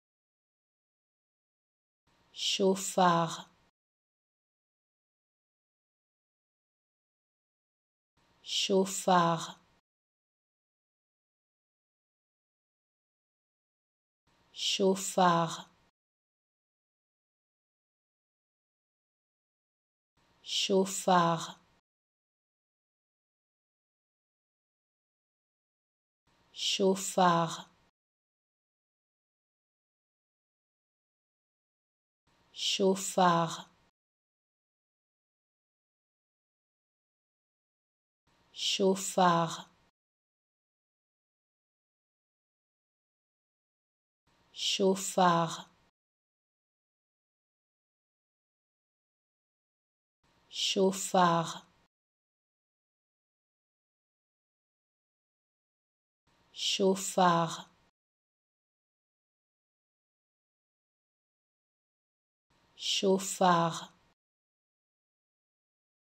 How to Pronounce ''Chauffard'' Correctly in French
Learn how to say and properly pronounce ''Chauffard'' in French with this free pronunciation tutorial. Apprenez à prononcer des mots en français grâce à nos tutoriels.